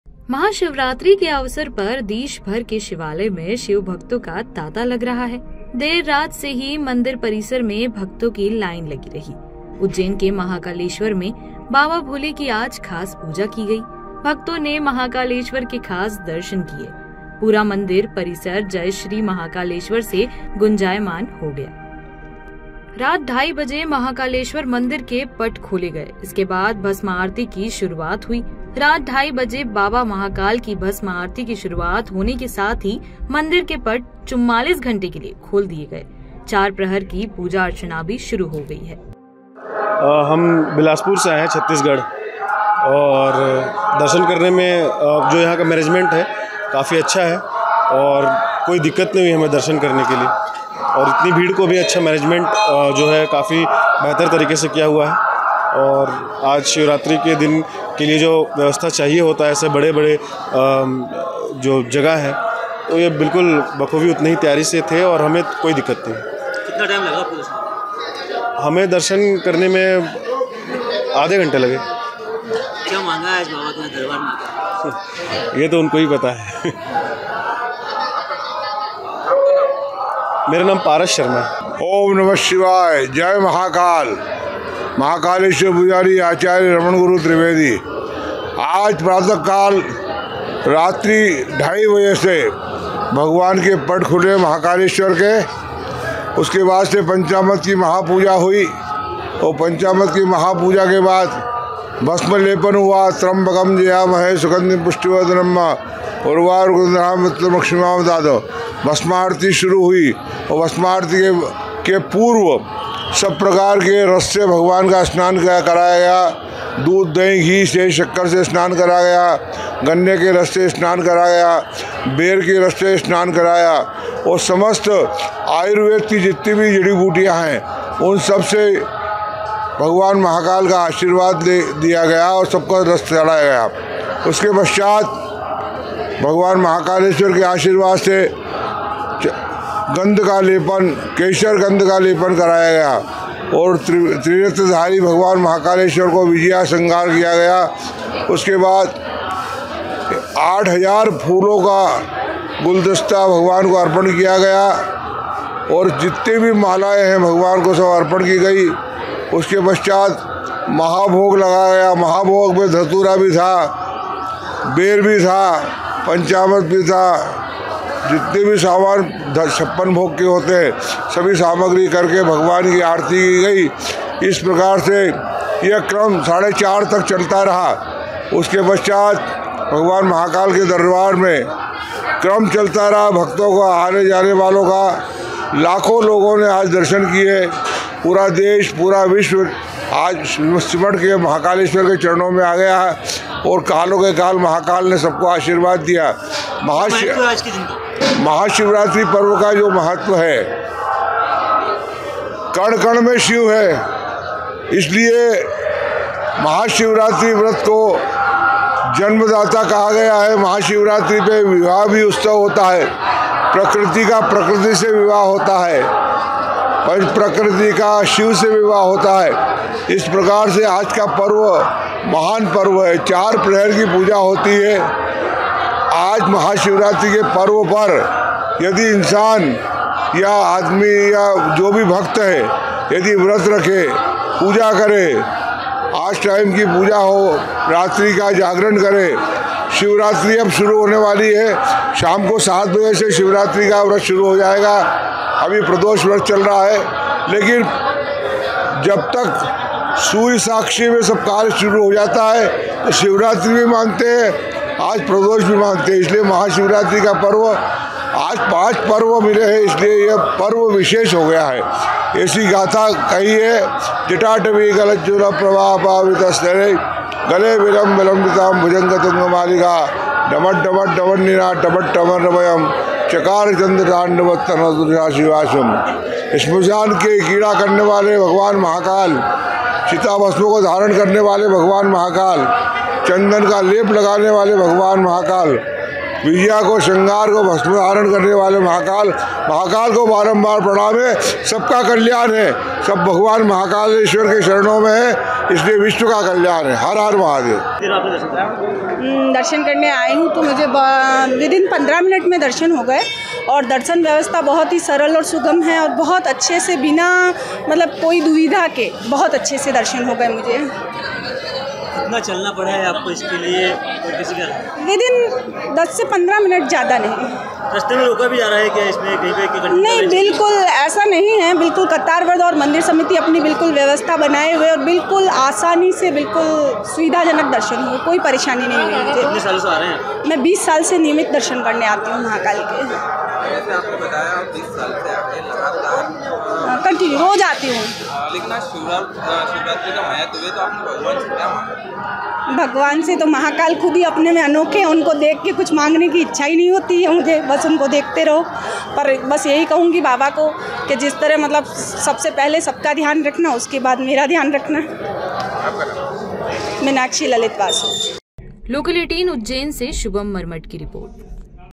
Mahakal Darshan 2024: महाकाल की नगरी में सज गई भोले नाथ की बारात, भक्तों की लगी कतार!
Mahashivratri 2024 : महाकाल की नगरी में सज गई भोले नाथ की बारात, भक्तों की लगी कतार!